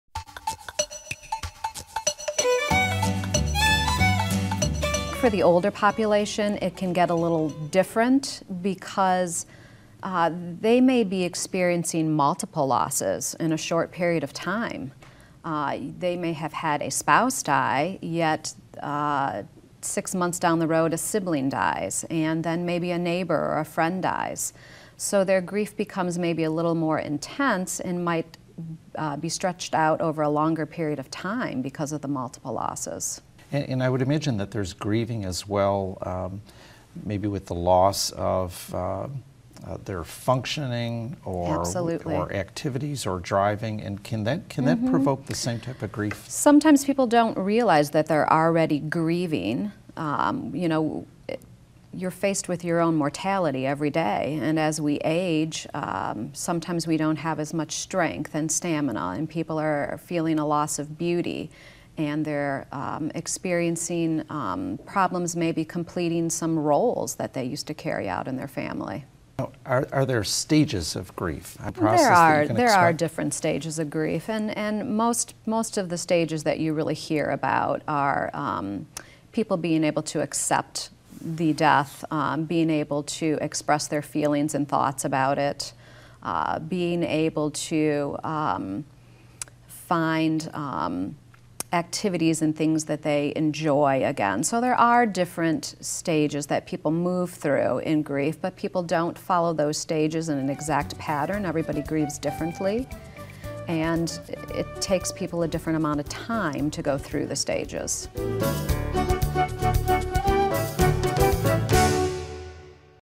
Grieving Is Personal
Grief takes many forms. Multiple deaths in your circle of life, loss of personal functions, and more generate grieving.  It's not uncommon to have life's events impact emotions. Understanding what may be occurring to yourself and those around you can have a material benefit.  Emotional reactions change and improve over time, or at least they should. Connie Doyle, a Bereavement Coordinator provides good insights for all in this video.